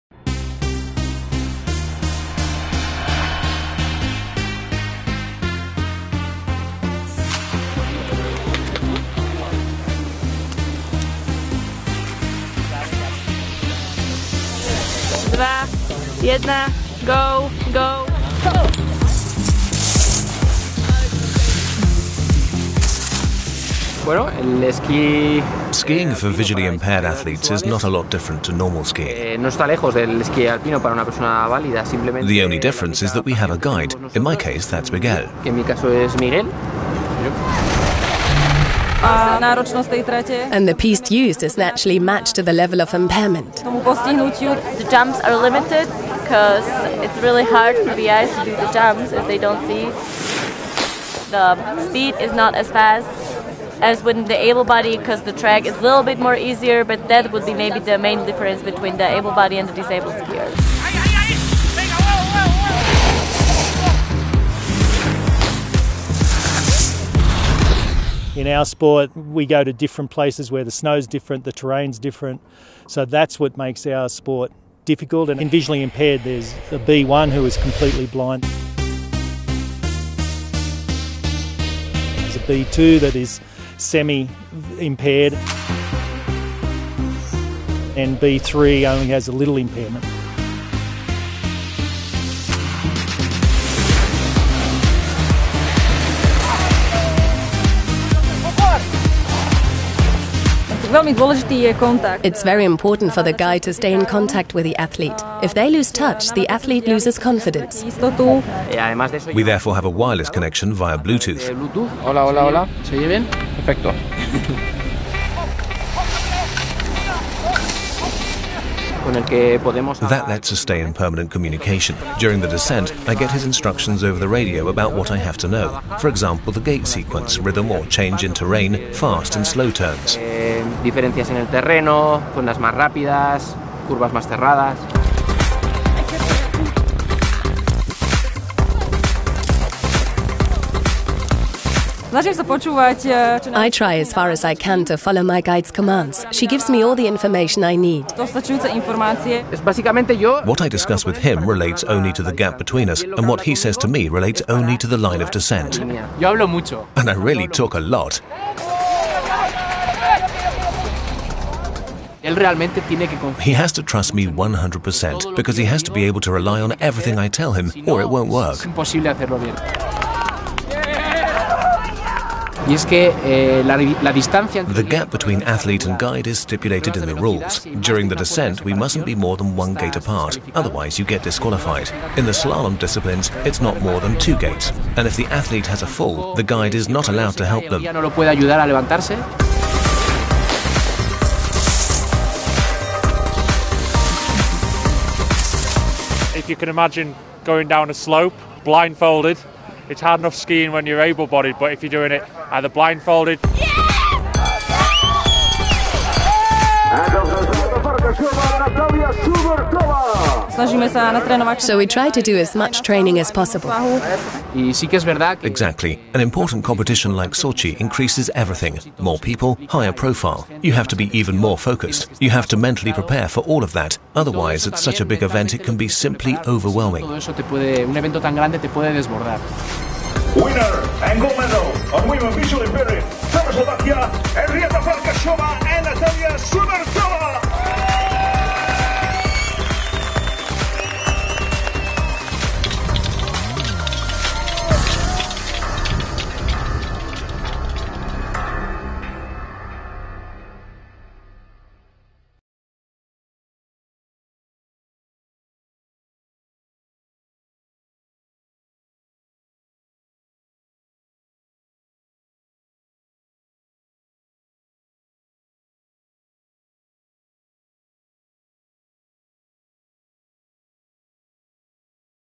How to para alpine skiing visually impaired category 720p
Discover what the para-alpine skiing - visual impairment category is
all about and which characteristics define this Paralympic discipline.
Experts and Paralympic athletes such as Jon Santacana and Henrieta
Farkasova explain the sport, their equipment, the relation with the
guide, the technique and the rules.